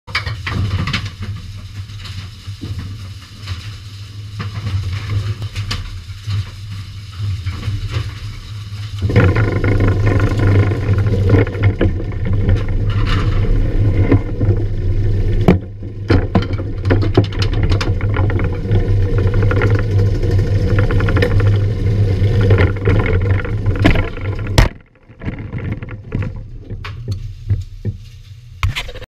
Close up of Candlepin Sweep
Close up of candlepin sweep at Alley Kat lane in Kingston MA